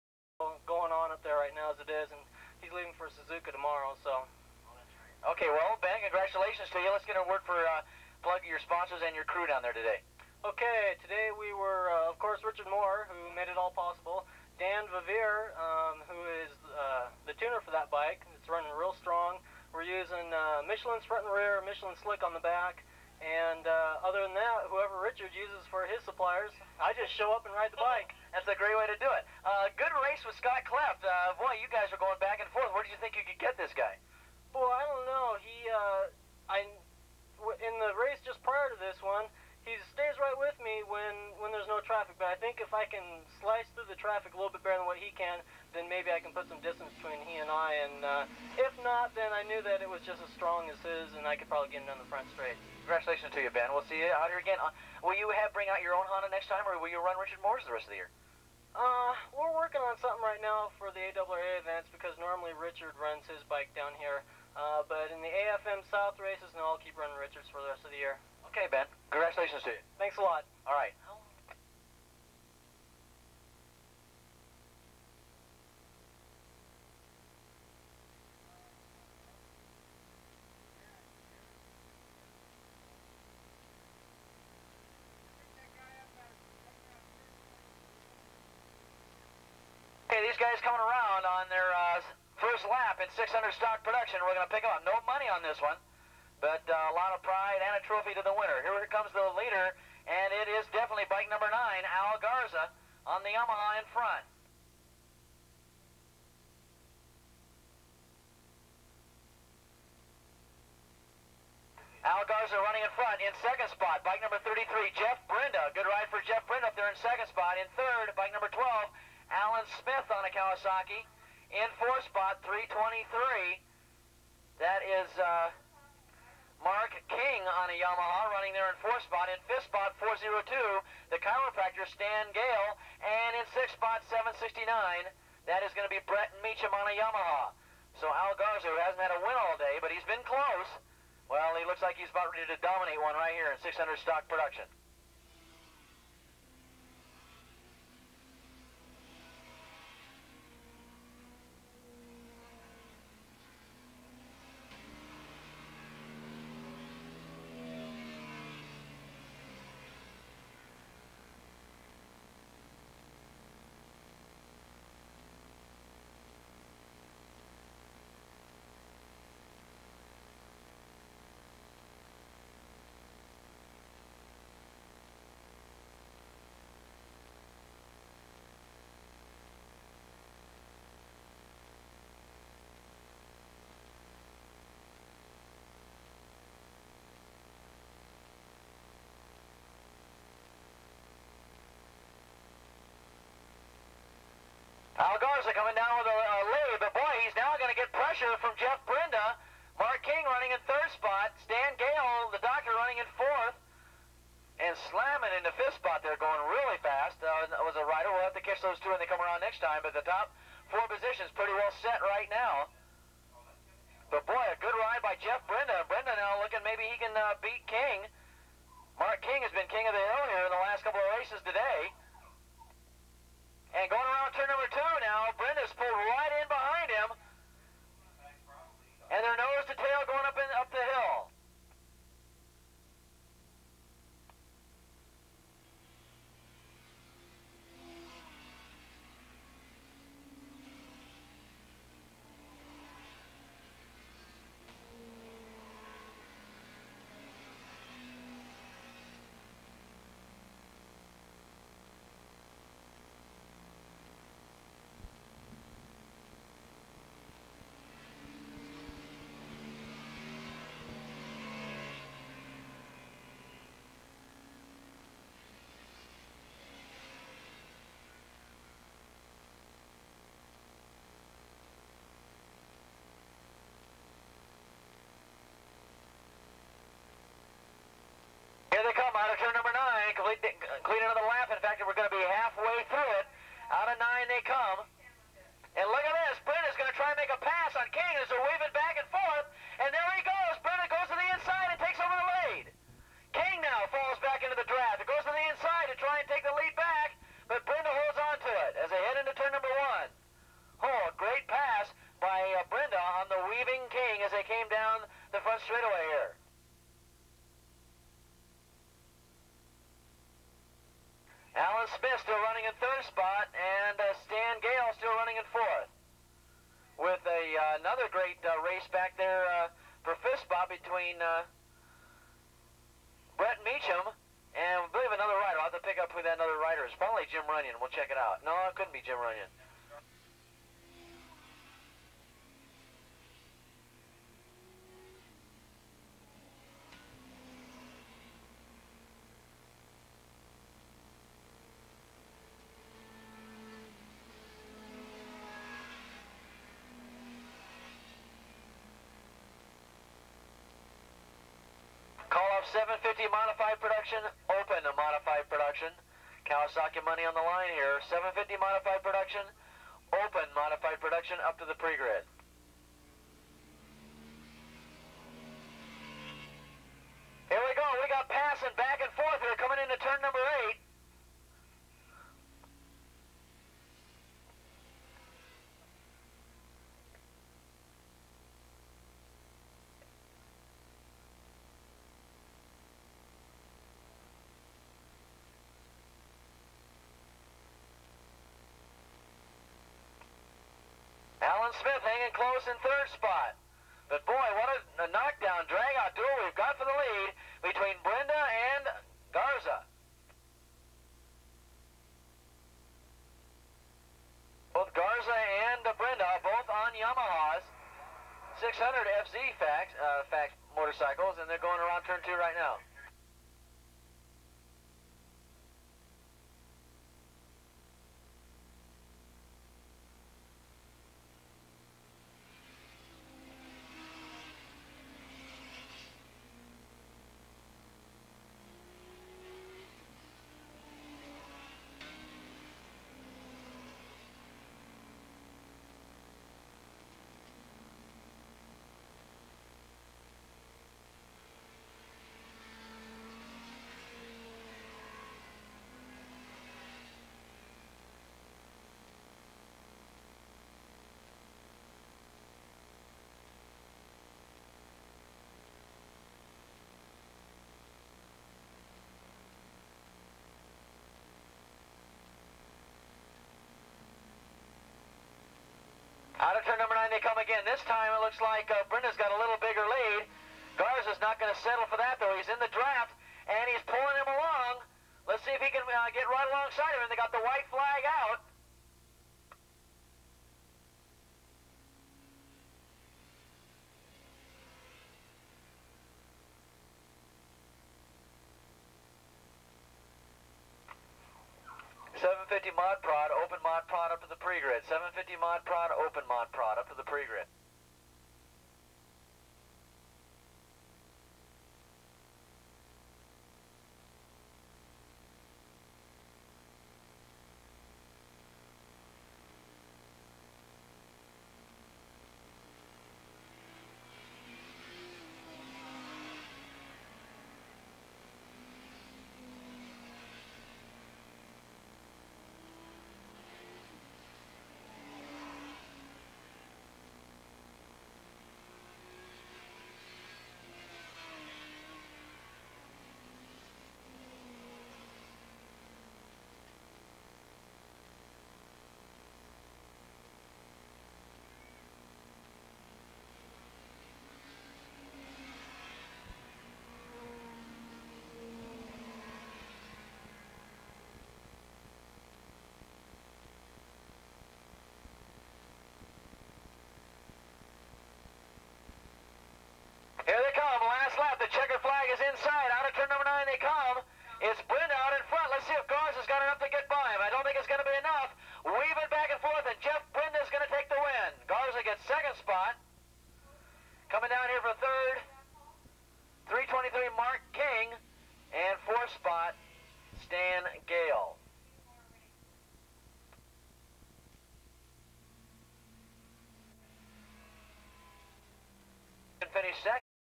ARRA 7/19/1987 600 Stock Production Willow Springs International Raceway
Stan Gale, Jeff Brenda, Mark King, Al Garza race for the win at ARRA 7/19/1987 600 Stock Production Willow Springs International Raceway